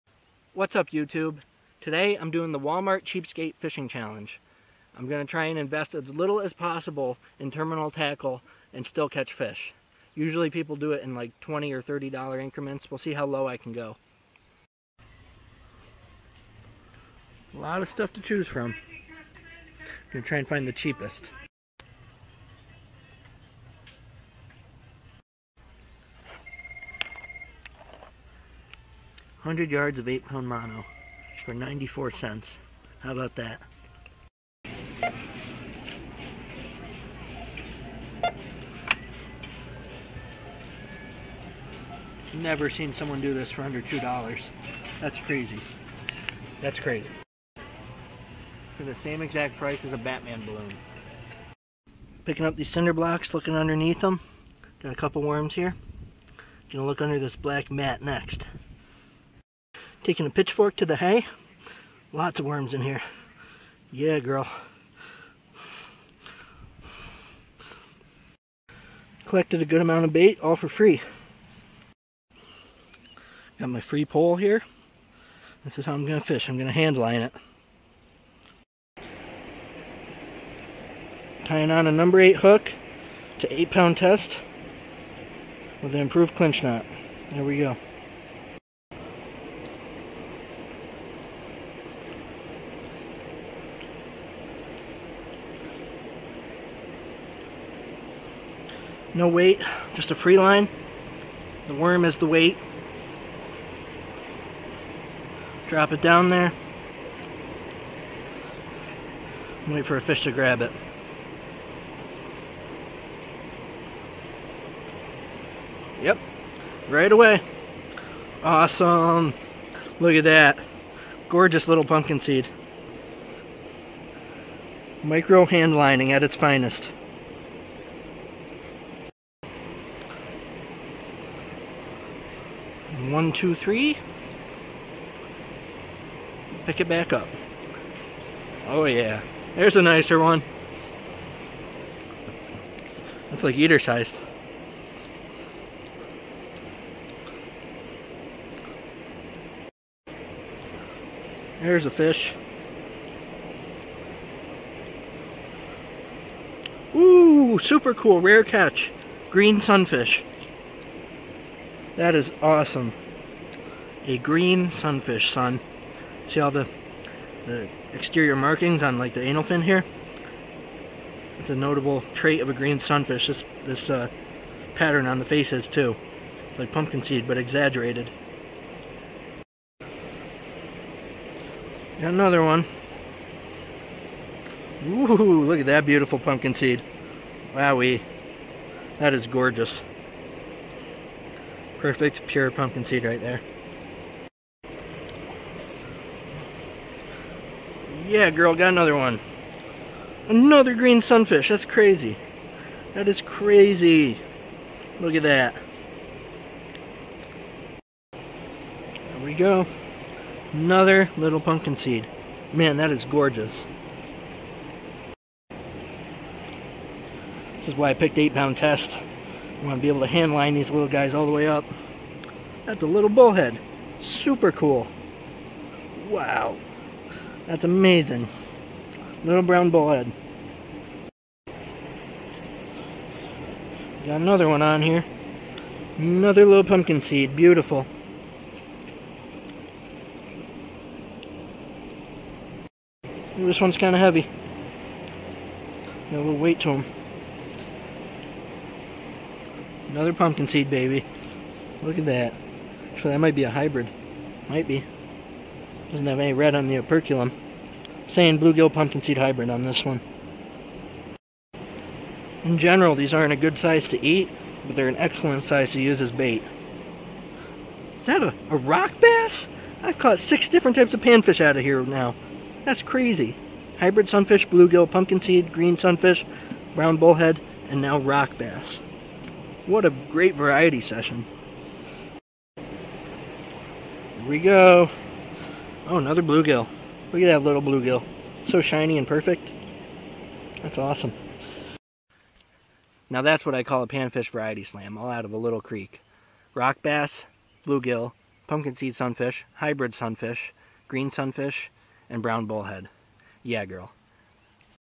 Vlog #69: $2 Walmart Fishing Challenge | Small Creek Panfish Slam
This is my sixty ninth vlog (video blog) filmed in July, 2018. In this video I attempt the cheapskate Walmart fishing challenge where I buy less than $2 of fishing tackle and use it to carry out a small creek (stream) panfish slam (green sunfish, bluegill, pumpkinseed sunfish, hybrid sunfish, brown bullhead, and rock bass) with worms as bait.